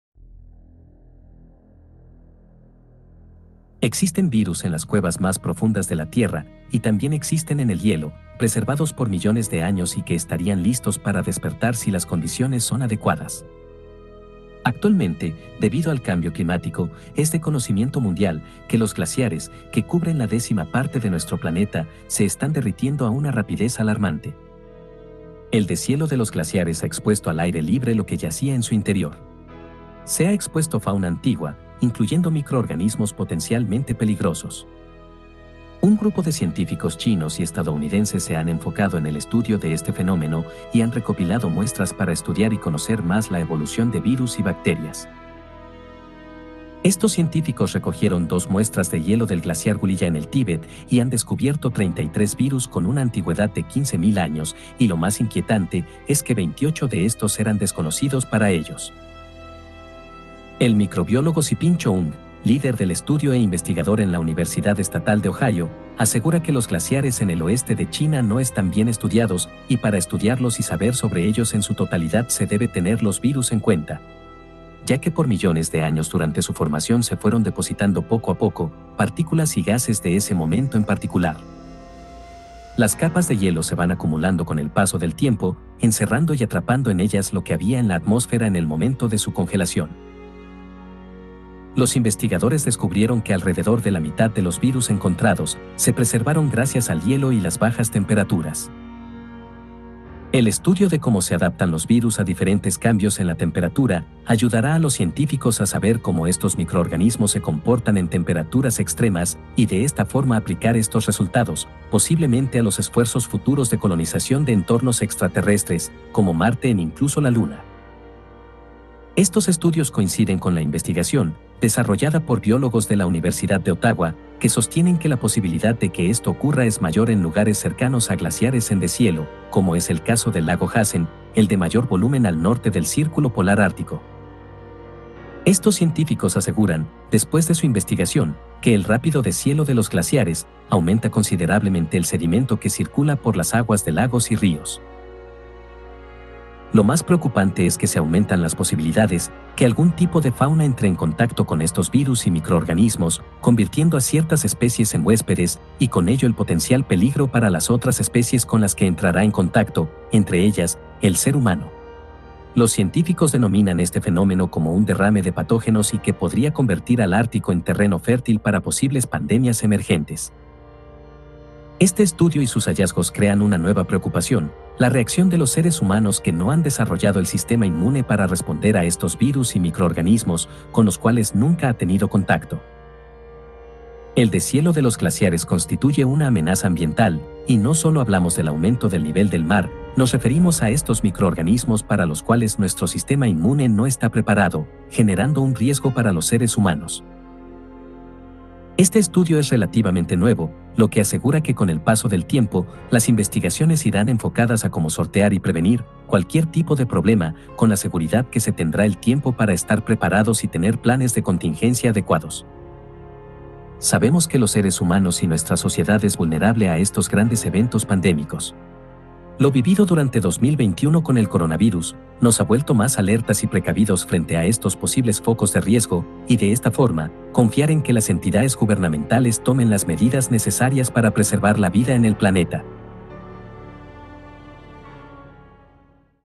¡VIRUS DE 15.000 AÑOS DE ANTIGÜEDAD PODRÍAN QUEDAR EXPUESTOS CON EL DESHIELO DE LOS GLACIARES!
Existen virus en las cuevas más profundas de la tierra, y también existen en el hielo, preservados por millones de años y que estarían listos para despertar si las condiciones son adecuadas.
Actualmente, debido al cambio climático, es de conocimiento mundial, que los glaciares, que cubren la décima parte de nuestro planeta, se están derritiendo a una rapidez alarmante.
El deshielo de los glaciares ha expuesto al aire libre lo que yacía en su interior. se ha expuesto fauna Antigua, incluyendo microorganismos potencialmente peligrosos.

Un grupo de científicos chinos y estadounidenses se han enfocado en el estudio de este fenómeno, y han recopilado muestras para estudiar y conocer más la evolución de virus y bacterias.
Estos científicos recogieron dos muestras de hielo del glaciar Guliya en el Tíbet, y han descubierto 33 virus con una antigüedad de 15 mil años, y lo más inquietante, es que 28 de estos eran desconocidos para ellos.

NASA’s Earth Observatory